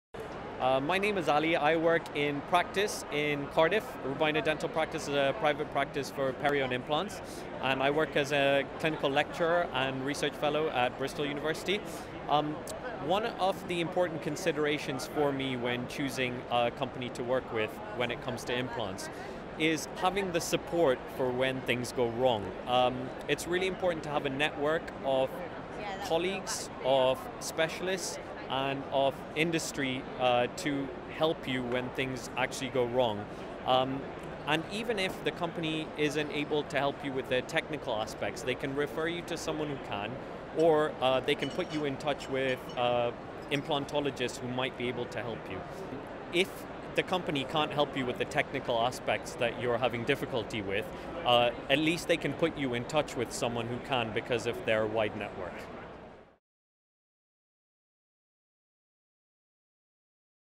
Dr Aly Virani, Dentist and Research Fellow in Dental Sciences and Lecturer, Bristol Dental Hospital
Dr Aly Virani, Dentist and Research Fellow in Dental Sciences and Lecturer at Bristol Dental Hospital expresses the "importance of having the support for when things go wrong, it's really important to have a network of colleagues and specialists".